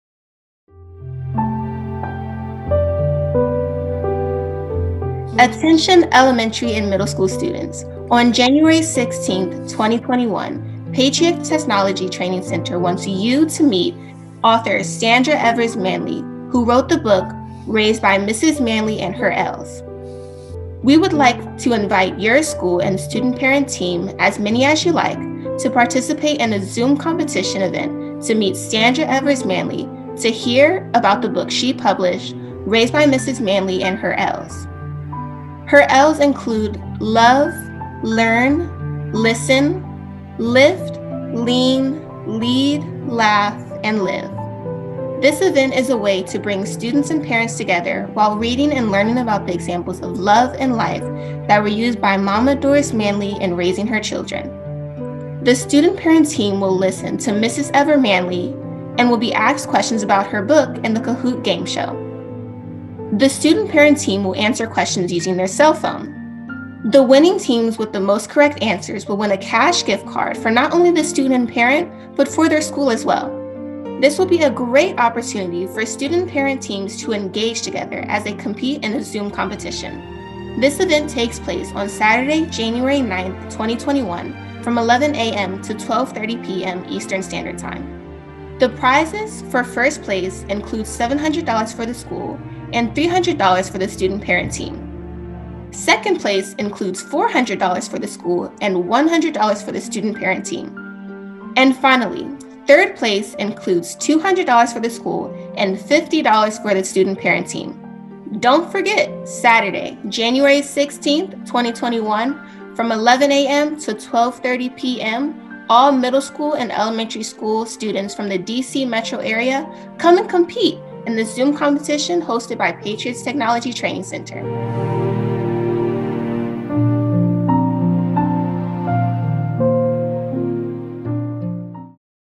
Patriots Technology Training Center
Patriots Technology Training Center provides (STEM) Science, Technology, Engineering, Math education to students in Prince George's County MD and beyond. The education is delivered via multiple programs throughout the calendar year which allows students to work in groups/teams and compete.